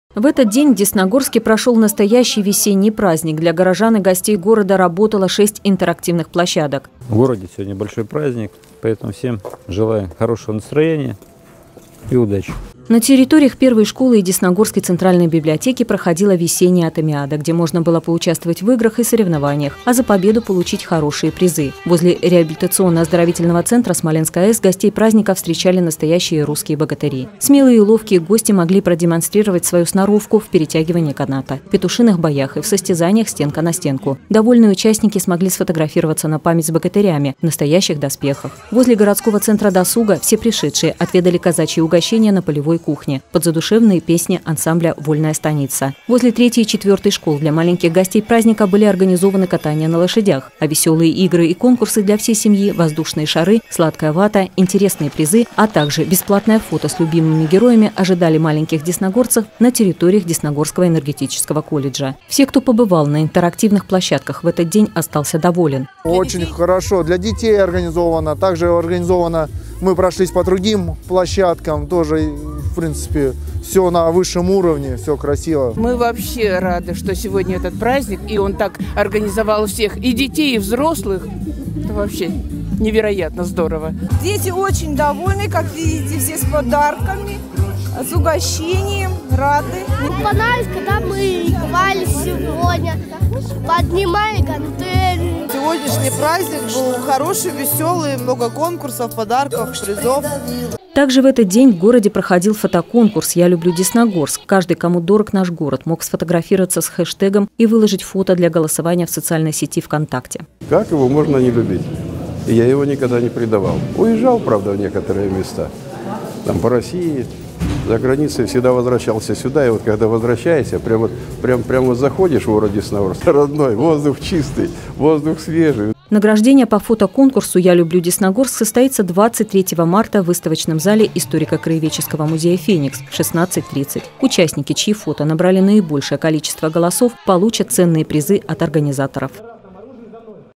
Десна-ТВ: Массовый праздник в городе
18 марта в городе прошёл массовый праздник, который был организован администрацией Десногорска при поддержке САЭС и профсоюзного комитета.